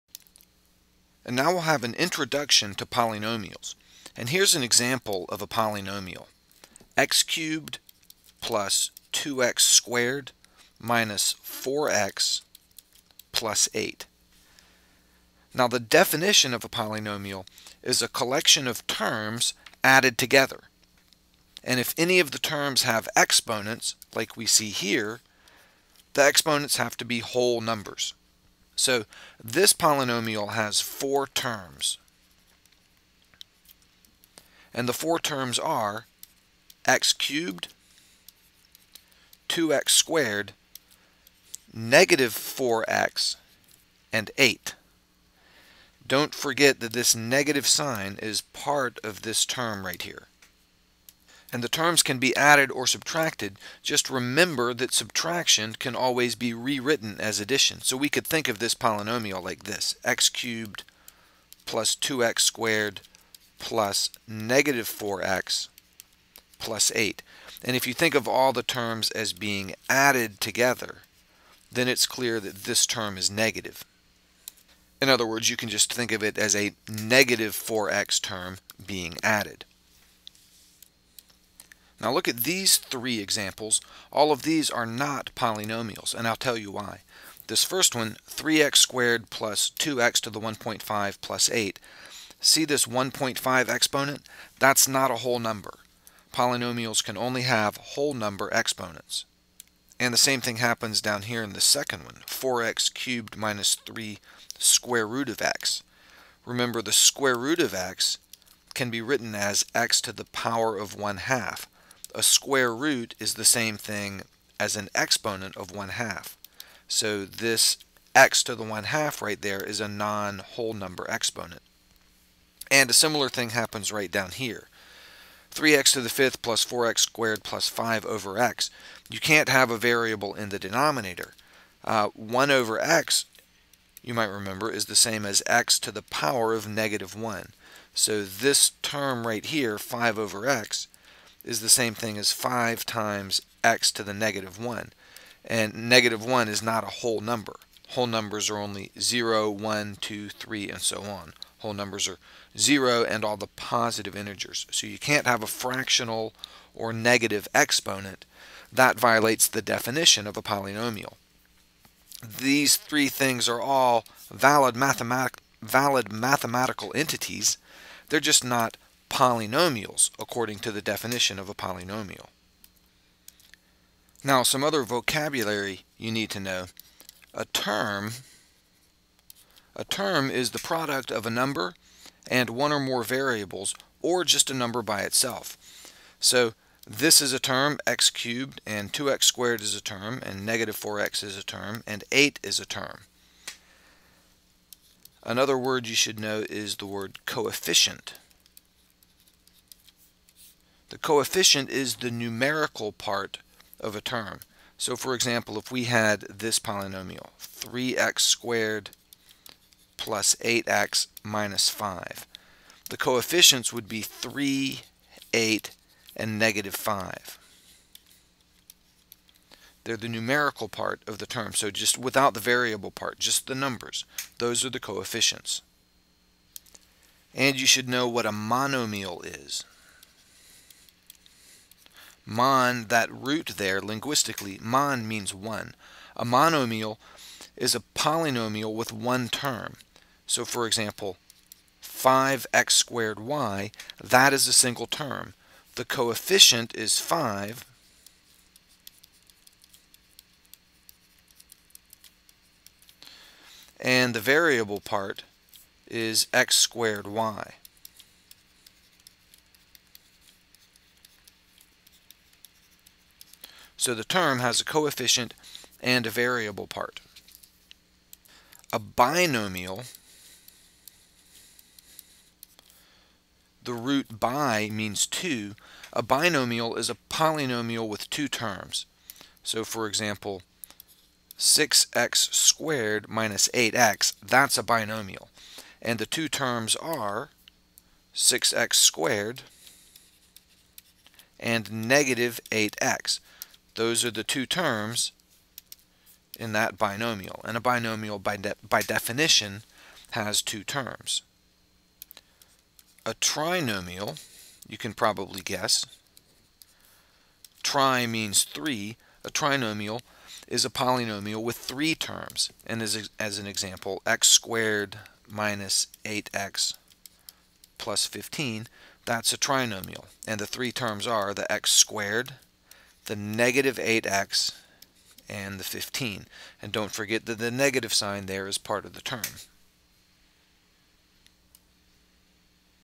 Prealgebra 10.1a - Introduction to Polynomials
An introduction to Polynomials.  From the Prealgebra course by Derek Owens, also appropriate for Algebra 1.